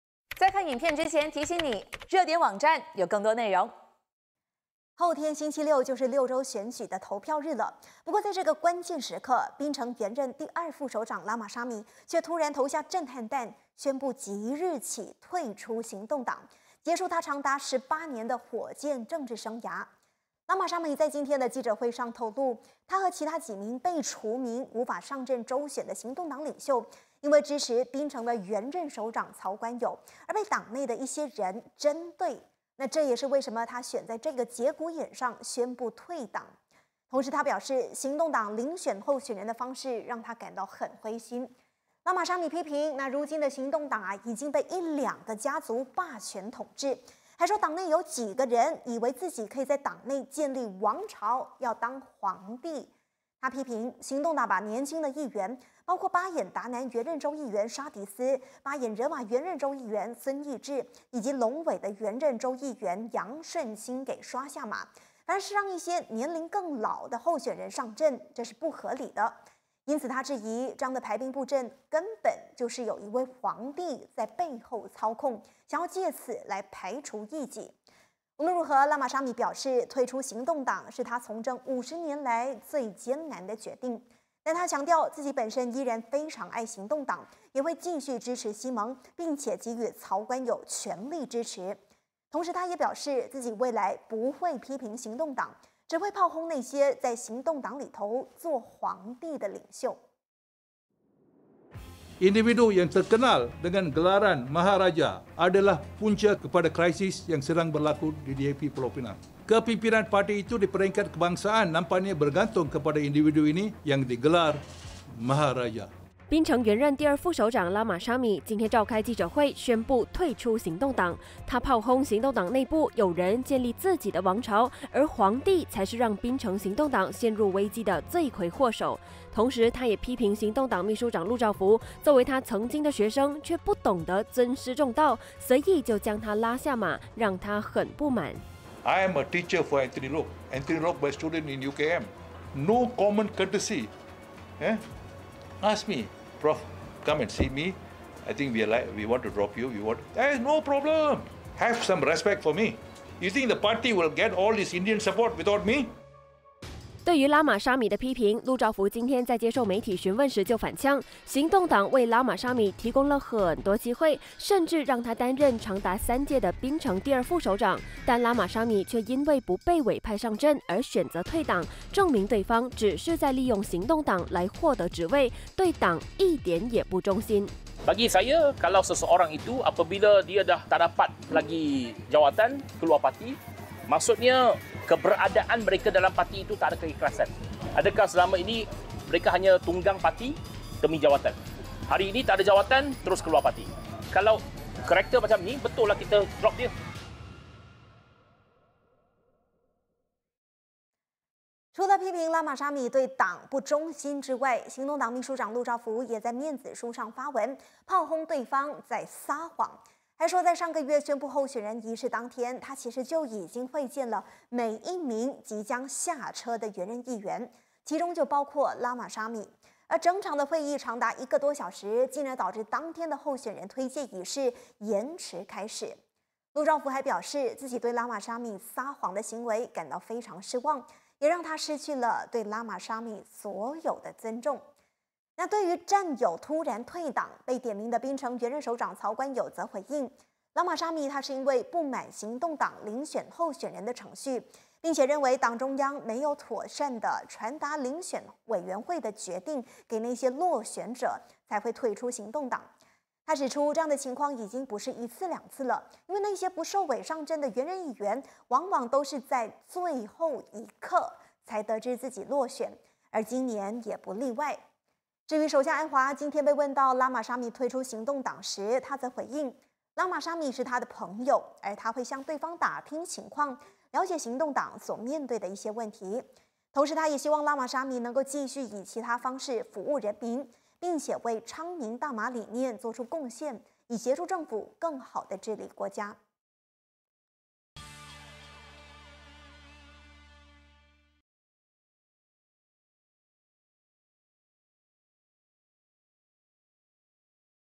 斥火箭内部霸权统治 拉马沙米退党抗议 | 八点最热报 10/08/2023
槟城原任第二副首长拉马沙米今天突然投下震撼弹，宣布即日起退出行动党，结束他长达18年的“火箭政治生涯”。拉马沙米在今天的记者会中透露，他和其他几名被除名，无法上阵州选的行动党领袖，因为支持槟城原任首长曹观友，而被党内一些人针对。